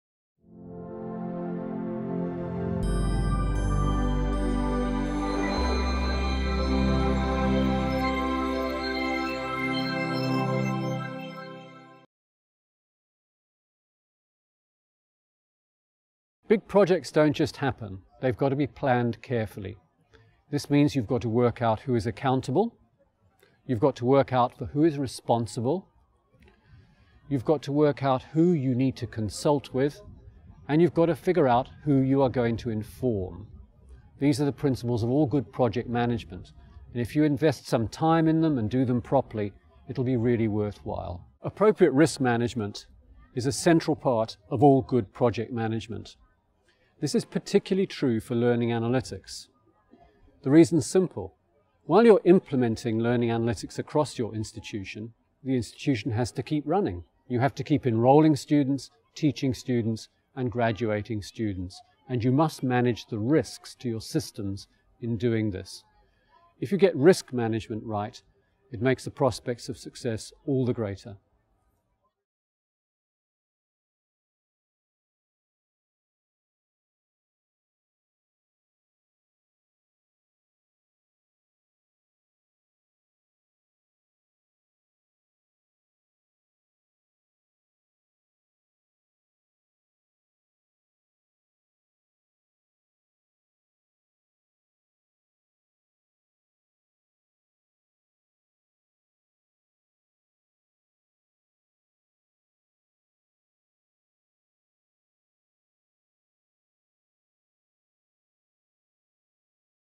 Jisc Learning Analytics: The need for project management
Video from the Jisc Learning Analytics CPD Modules
Module 3 - Institutional Readiness
Topic 5: An alternative approach to project management
Title: The need for project management
Presenter: Martin Hall